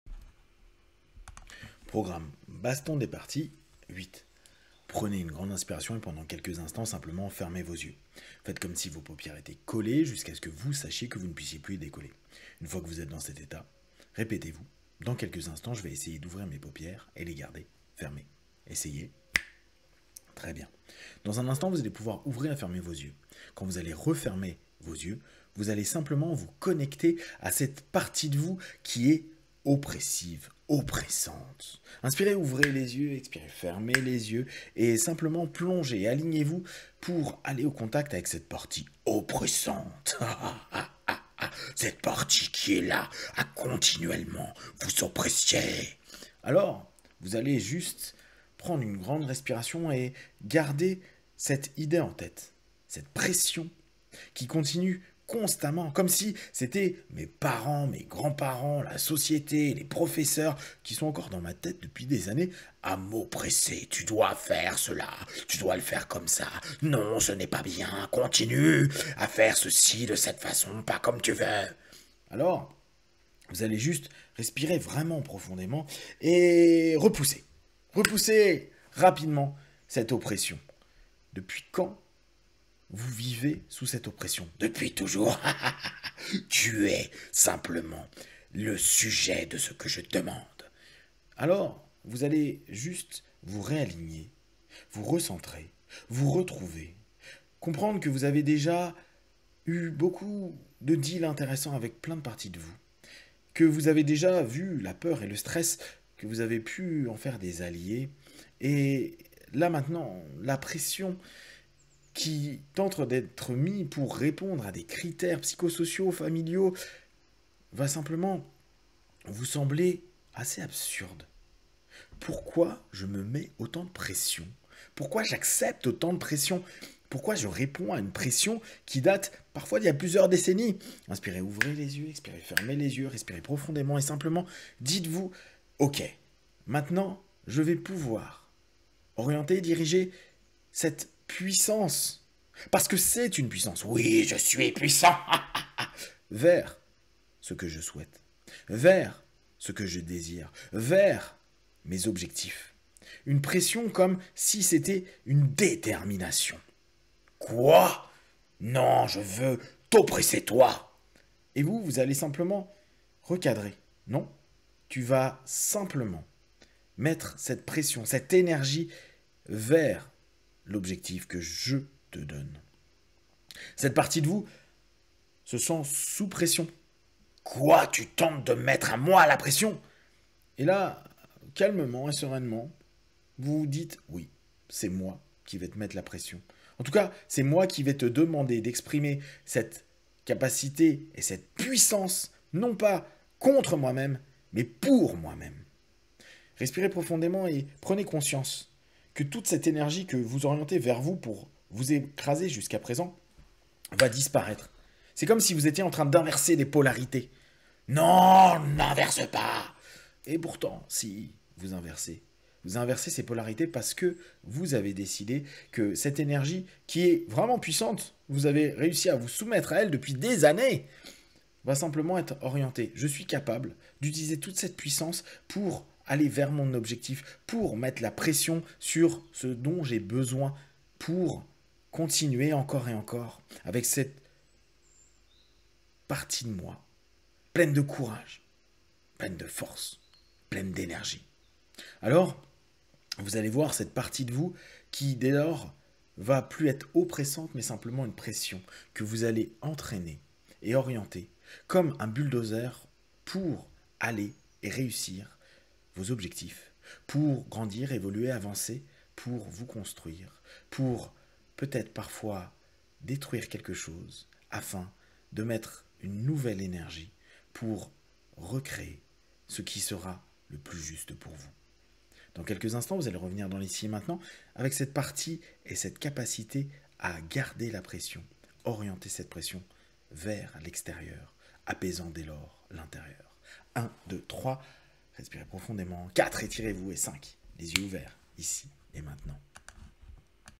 HnO Mp3 Hypnose #819 : Thérapie des parties #15 / Baston des parties #8 : Oppression (110422)
Une nouvelle série pour travailler quotidiennement avec des parties de vous et les recadrer pour en faire des facettes constructives de vous mêmes.

Attention les audios Thérapie des parties sont ... particuliers :)

Be One
Pank